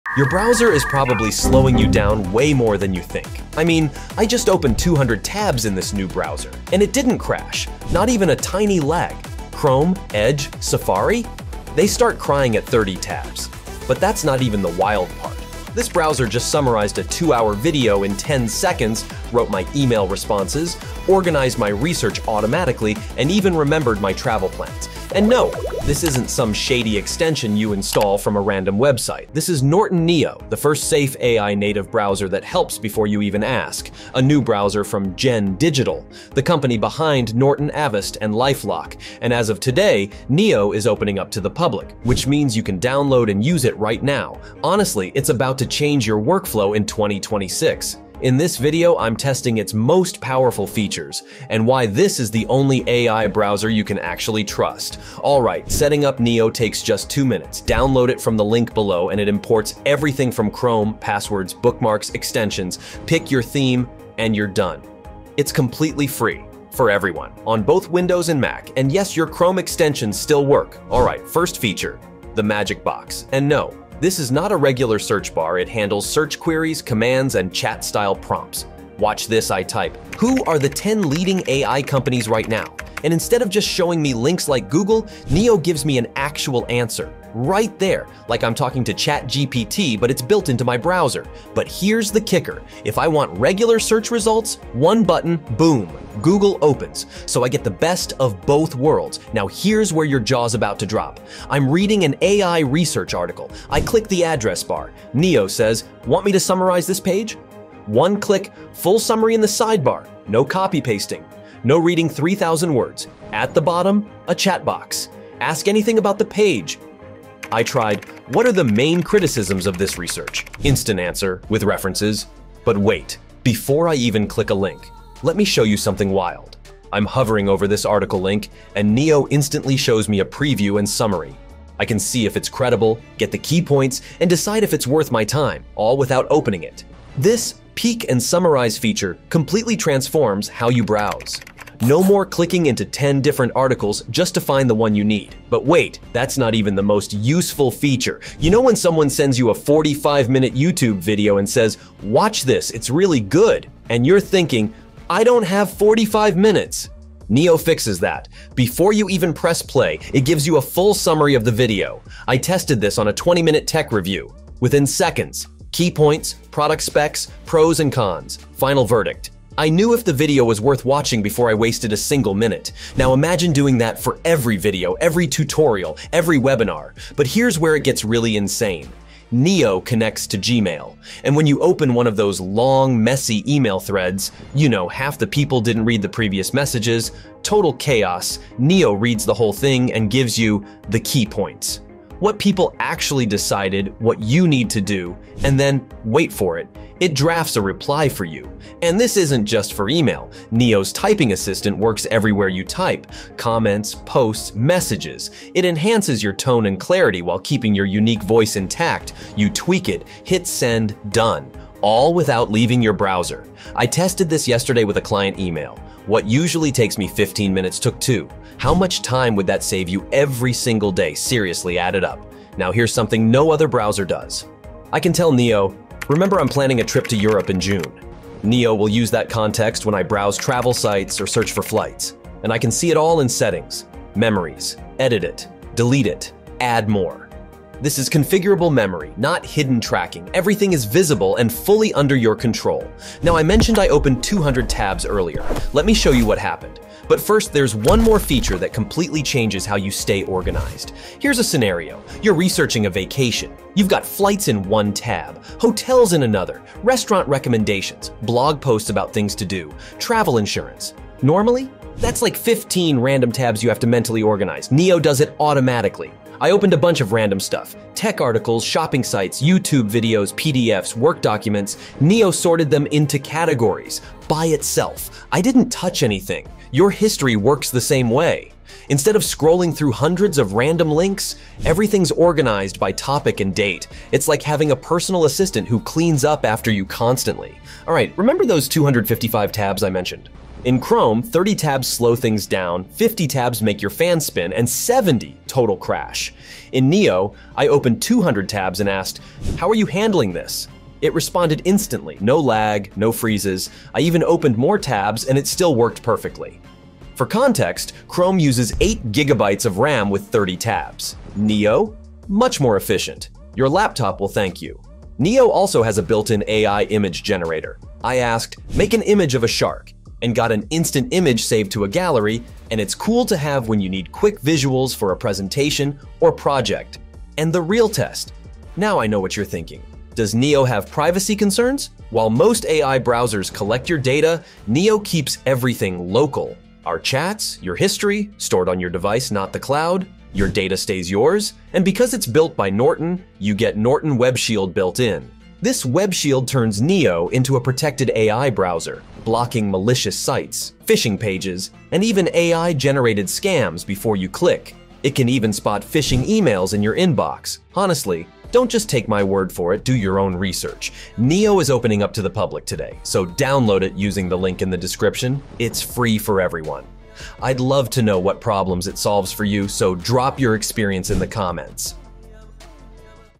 Chrome Users Are DITCHING Google for THIS in 2026
🌐 CHROME USERS NEED TO SEE THIS
Tired of your browser crashing with too many tabs? I tested Norton Neo - the first safe AI-native browser - and compared it to Chrome and Edge using real-world tasks.

⏱️ TIMESTAMPS:
0:00 - Introduction & The Browser Problem
0:25 - What is Norton Neo?
0:55 - Setting Up Neo (2 Minutes)
1:11 - Magic Box AI Command Center
1:41 - Page Summarization Feature
2:03 - Peek & Summarize (Link Preview)
2:28 - YouTube Video Summarization
2:59 - Gmail Integration & Email Summaries
3:21 - AI Typing Assistant
3:42 - Smart Memory Features
4:03 - Auto Tab Organization
4:48 - 200 Tab Stress Test
5:15 - Built-in AI Image Generator
5:29 - Privacy & Security (Norton WebShield)
5:58 - Final Verdict & Download

🎯 WHO THIS IS FOR:
- Chrome users frustrated with crashes and slow performance
- Anyone who keeps 20+ tabs open daily
- Researchers who need to summarize articles quickly
- Content creators managing multiple projects
- Privacy-conscious users wanting built-in protection
- Anyone curious about AI-powered browsing

❓ COMMON QUESTIONS:

Q: Is Norton Neo really free?
A: Yes, completely free for everyone on Windows and Mac.

Q: Can I import my Chrome bookmarks and passwords?
A: Yes - Neo imports everything from Chrome in about 2 minutes.

Q: Do Chrome extensions work in Neo?
A: Yes, Neo is Chromium-based so your extensions work normally.

Q: Is my data safe with an AI browser?
A: Neo stores data locally on your device, not in the cloud. Plus Norton WebShield provides built-in security.

Q: How does the 200 tabs without crashing work?
A: Neo uses efficient memory management - in my testing it used far less RAM than Chrome with the same tabs.

Q: Why do you recommend Neo over other AI browsers?
A: Beyond sponsorship - it combines AI features with Norton's security expertise. Other AI browsers often lack proper protection.

All testing and comparisons based on my genuine experience using real-world tasks. Your results may vary based on hardware and use case.